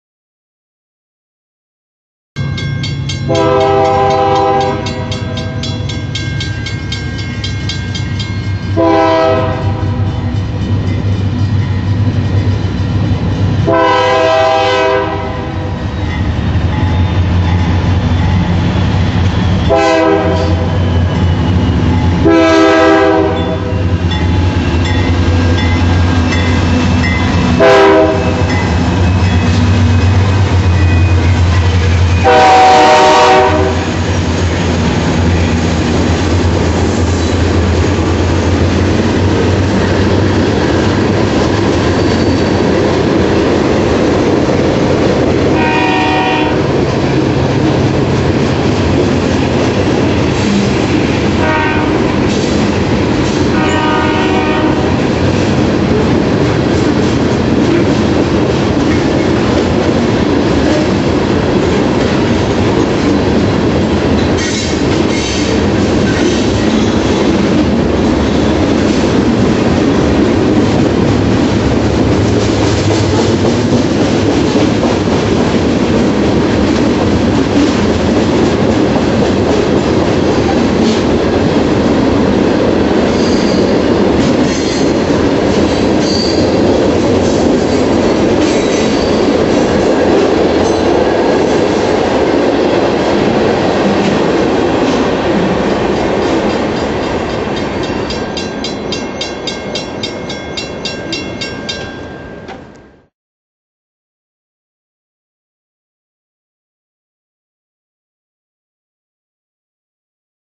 UP 9375
UP rock train in Pine Bluff, Arkansas. Taken 2/8/09